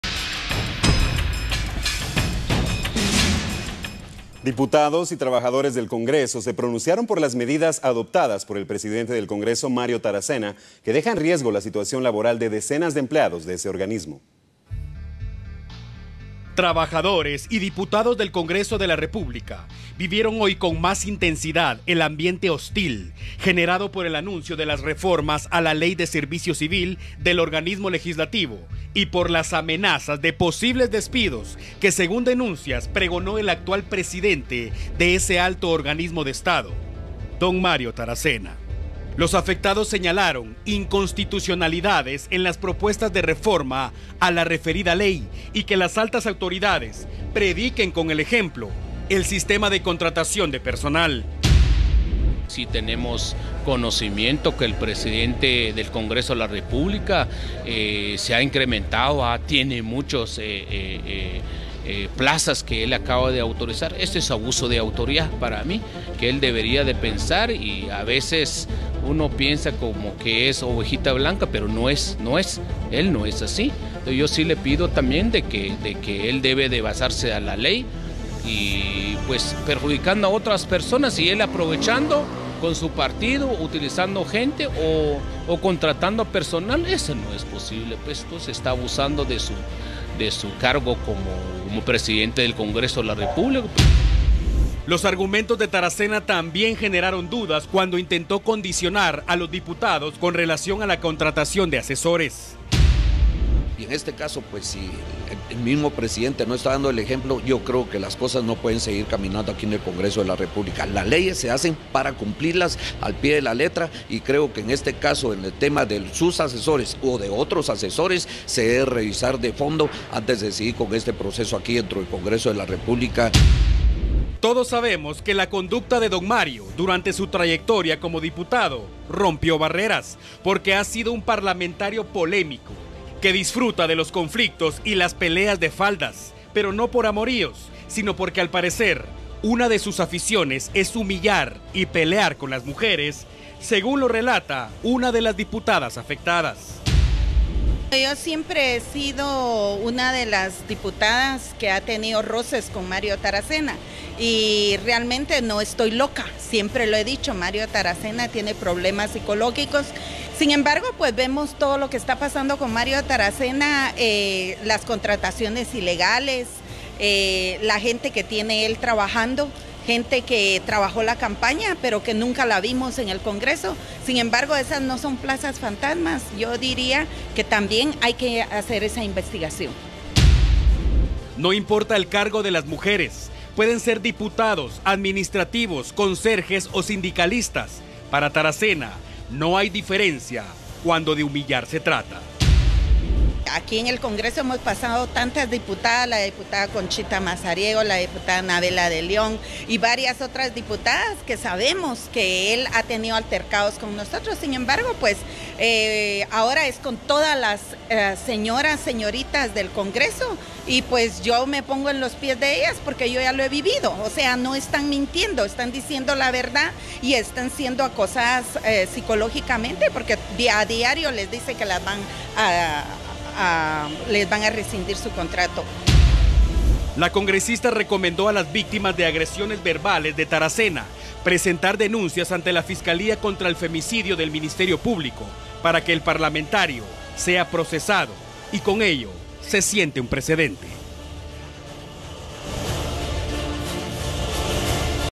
Trabajadores del Congreso denunciaron presiones de Mario Taracena
Diputados y trabajadores del Congreso se pronunciaron por las medidas adoptadas por el presidente, Mario Taracena. Estas dejan en riesgo la situación laboral de decenas de trabajadores de ese organismo. Señalan que a Taracena le gustan las peleas de faldas.